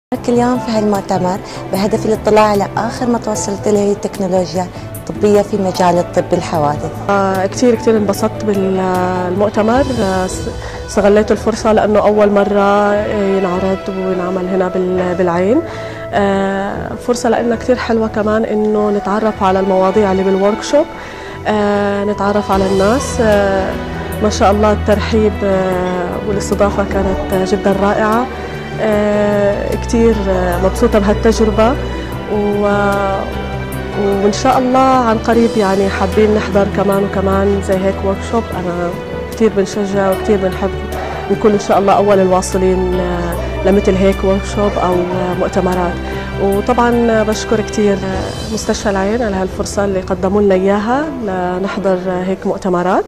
مشاركة منطقة الظفرة في مؤتمر صحة الأول لما هو أبعد من الاسعافات المتقدمة بتنظيم من مستشفى العين
مشاركة منطقة الظفرة في مؤتمر صحة الأول لما هو أبعد من الاسعافات المتقدمة بتنظيم من مستشفى العين إحدى منشآت شركه_صحه Al Dhafra Region has participated in “1st Conference Beyond ATLS” that was organized by Al Ain Hospital
Specialized Care for the Trauma Patient Thoracic Vascular & Orthopedic Wet-Lab ☎️80050